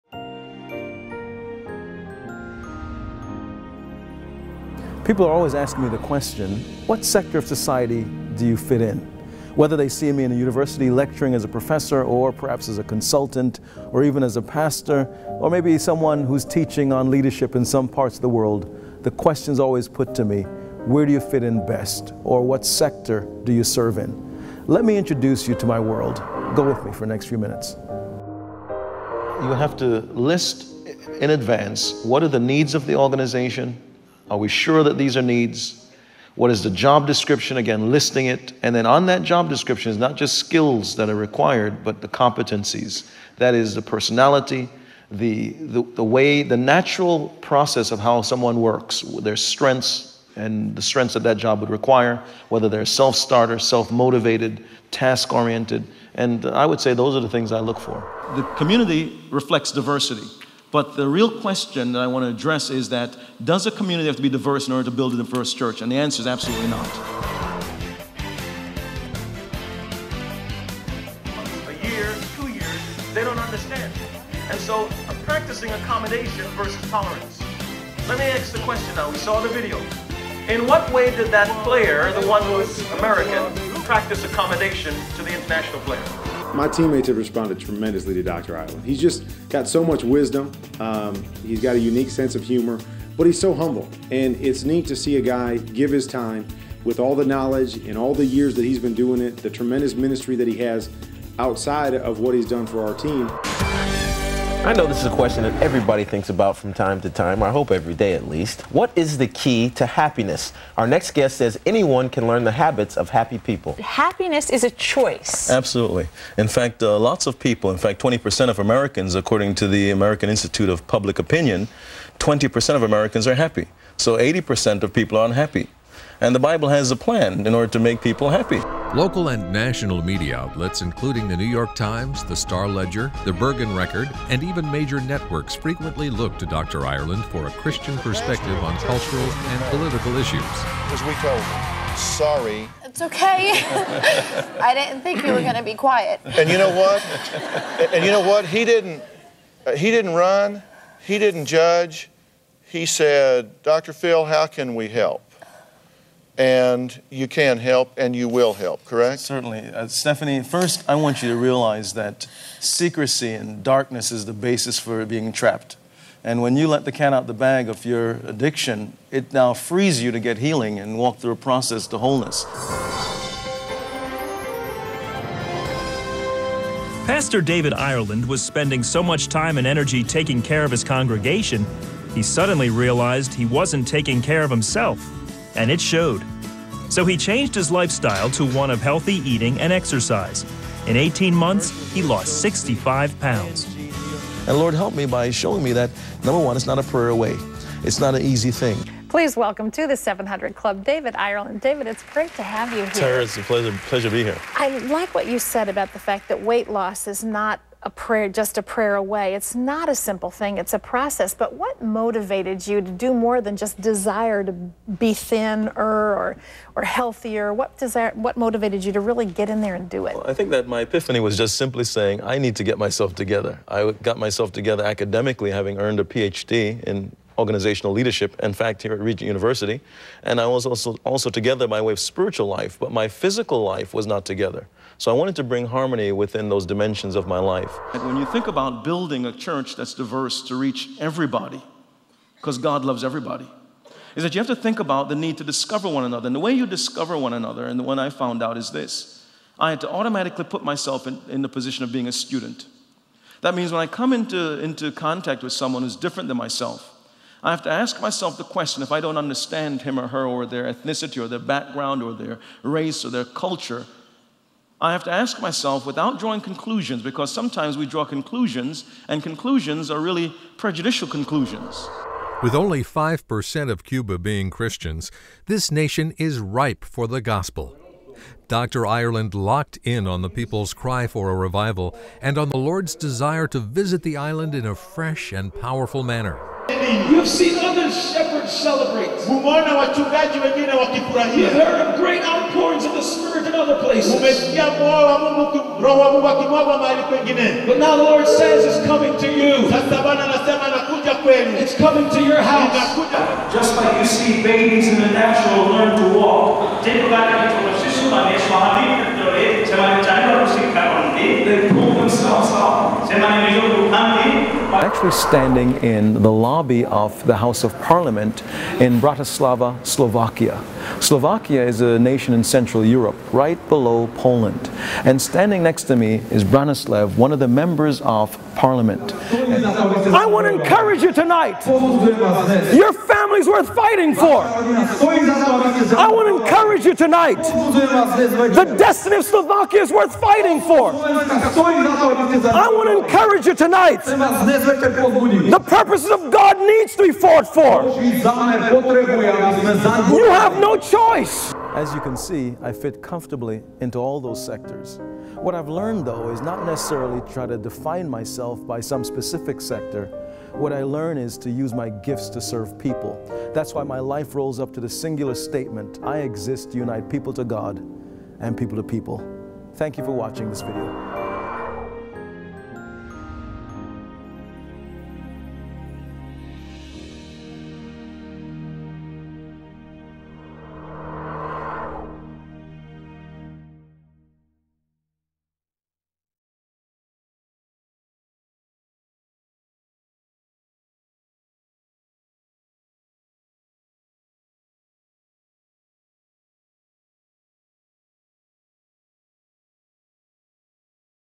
The World of David D. Ireland, Ph.D.
I am founder and senior pastor of Christ Church, a thriving 7,000-member multisite and multiracial congregation in northern New Jersey.

My personal mission is to be an authentic reconciler. I live to actualize the Bible's mandate that the church operates inter-culturally and functions as a transforming institution. I take this mission beyond the church walls by serving as diversity consultant to the National Basketball Association.

For nearly 25 years, I have coached leaders all over the world in organizations, churches, government agencies, and universities. This vast breadth of leadership experience is the greatest gift I can offer you.

2013 Version 2